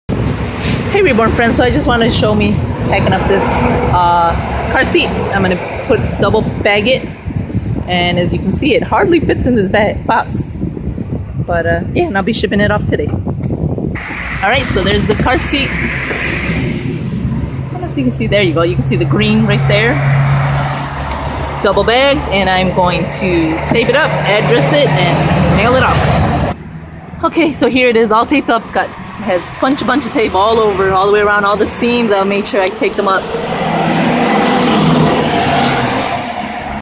Reborn Carseat box packing
I double bagged the carseat and taped it up alot !!!